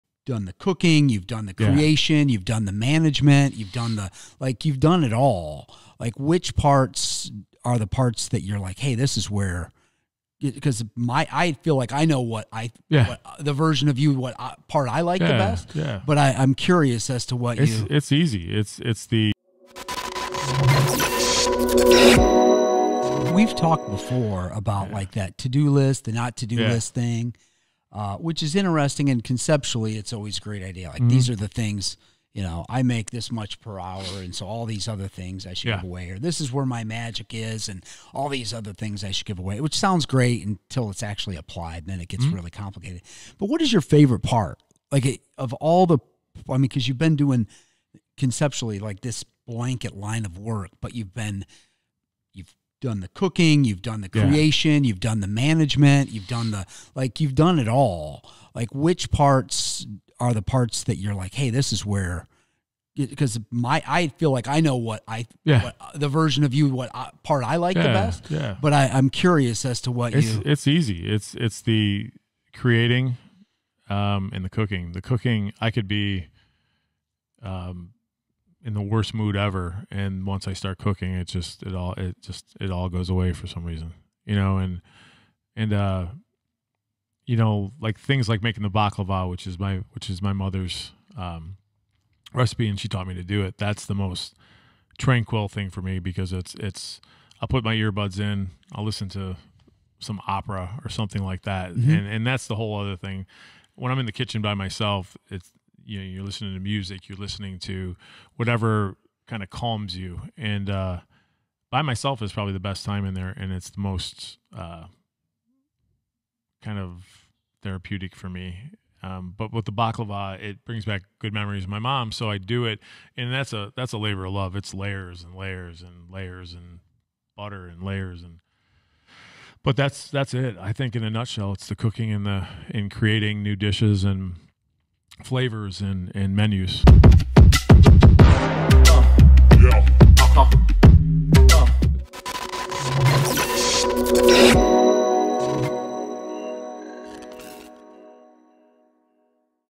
Hey Moussa, what do you love the most about your job?
Dan DeVerna Podcast Episode 25 - Moussa Salloukh, Owner of Souk